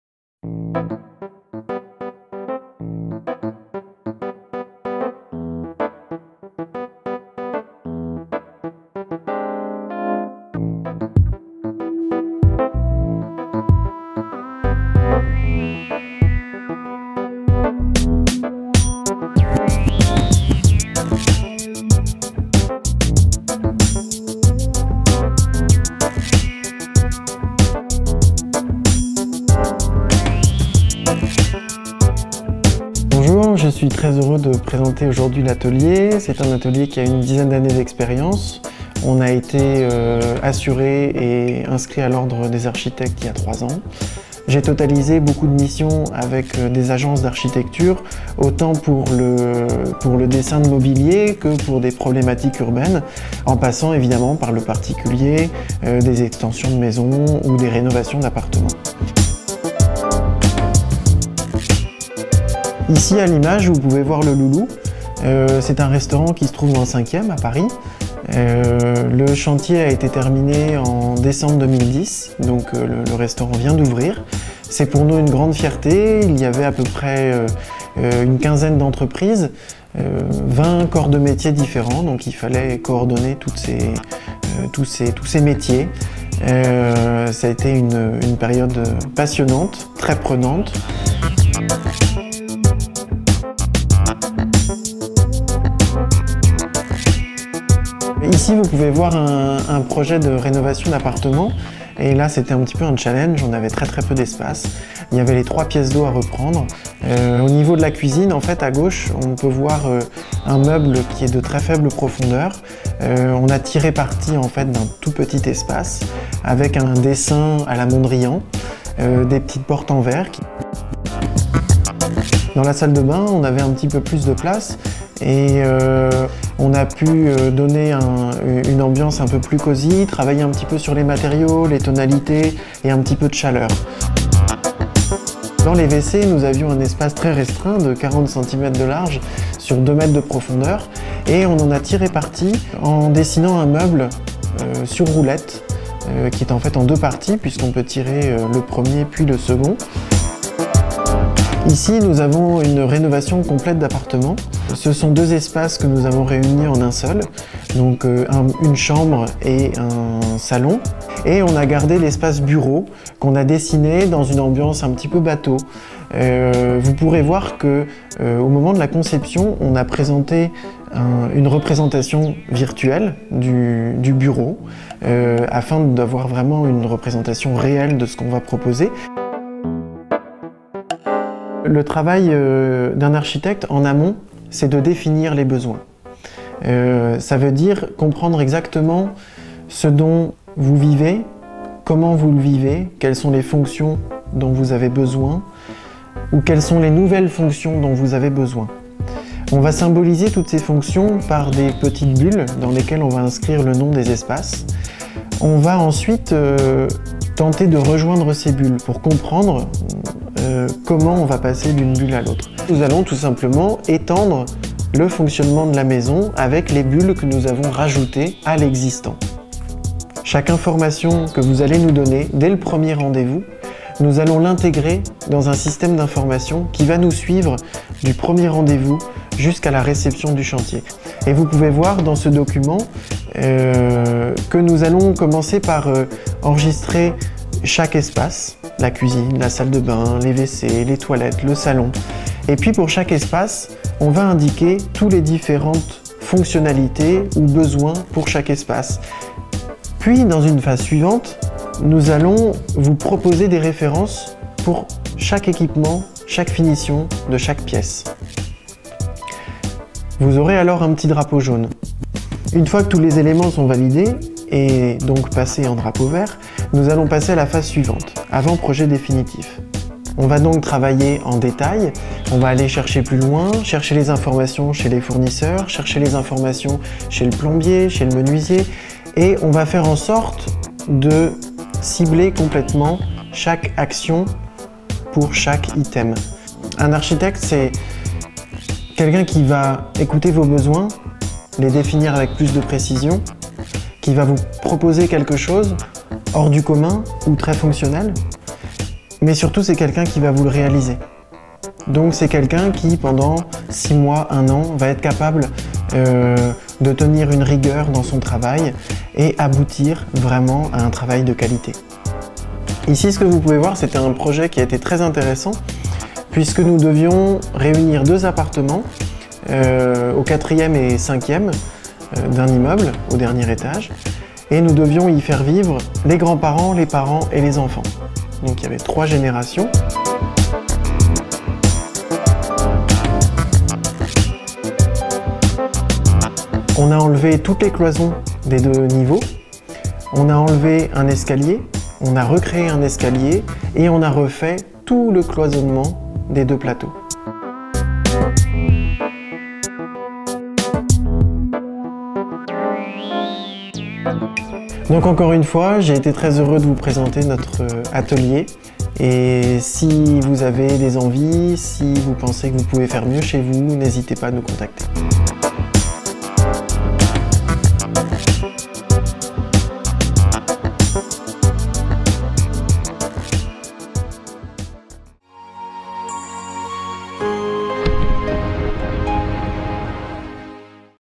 Atelier d\'architecte - Les Pavillons sous Bois
Atelier Instant T est un atelier d\\\'architecte situé à Les Pavillons sous Bois près de Bobibny en Seine St Denis.